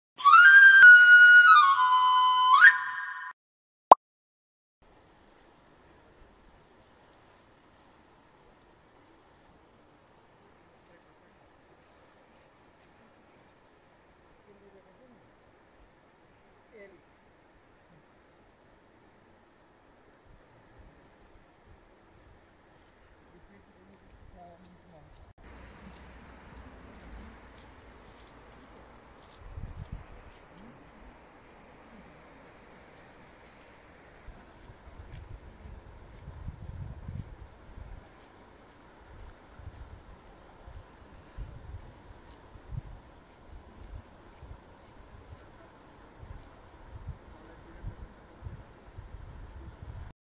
Una persona desafía a Justine navegando por el Miño con una brca
Una persona decidió esta tarde salir a navegar con su barca hinchable por el Miño, lo que no está permitido. Además, lo hace en pleno temporal en la ciudad.